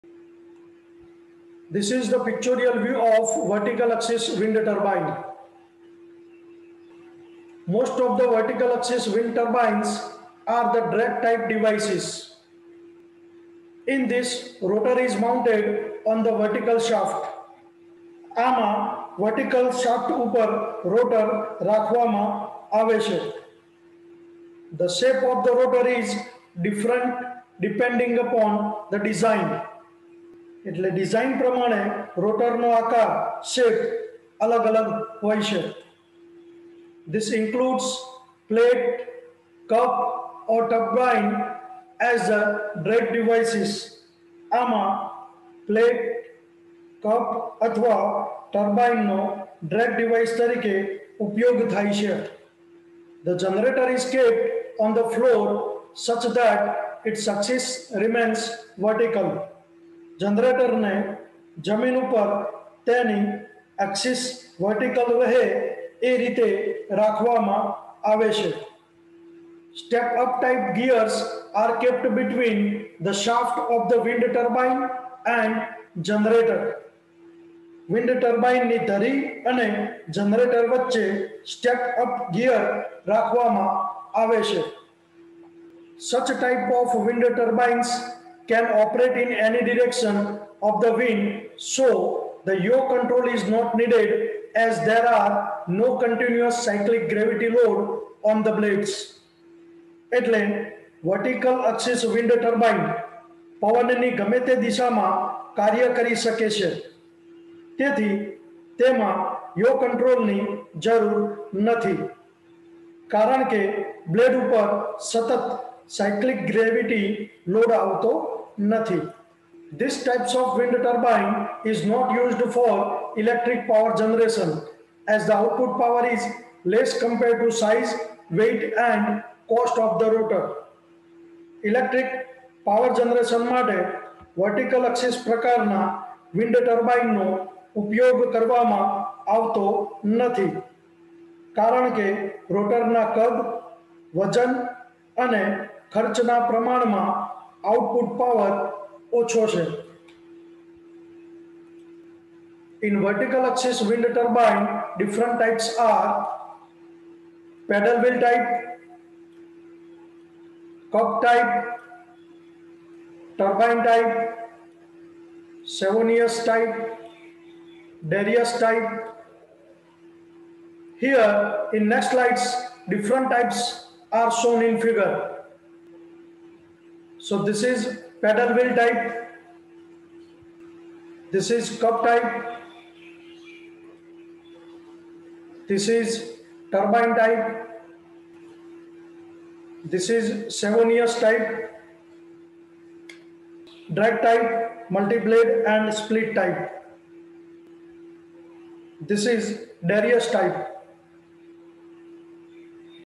Vertical Axis Wind Turbine VAWT
In Vertical Axis Wind Turbine VAWT different types are:-

Paddle wheel type
Cup type
Turbine type
Savonius type
Darrieus type

Wind Duration Curve, Height from ground and wind velocity, Relation among wind velocity-air mass and wind power

Reference:
EPG Book of Atul Prakashan